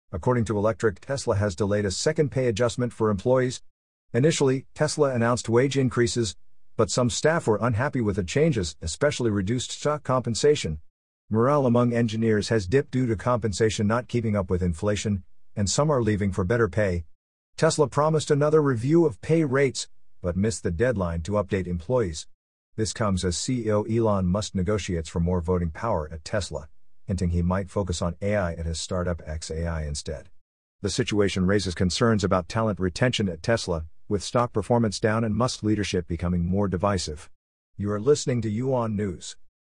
"Tesla Employee Pay Adjustment Delay Sparks Unrest"
...Source Transcript: According to Electrek, Tesla has delayed a second pay adjustment for employees. Initially, Tesla announced wage increases, but some staff were unhappy with the changes, especially reduced stock compensation. Morale among engineers has dipped due to compensation not keeping up with inflation, and some are leaving for better pay. Tesla promised another review of pay rates, but missed the deadline to update employees. This comes as CEO Elon Musk negotiates for more voting power at Tesla, hinting he might focus on AI at his startup xAI instead. The situation raises concerns about talent retention at Tesla, with stock performance down and Musk's leadership becoming more divisive.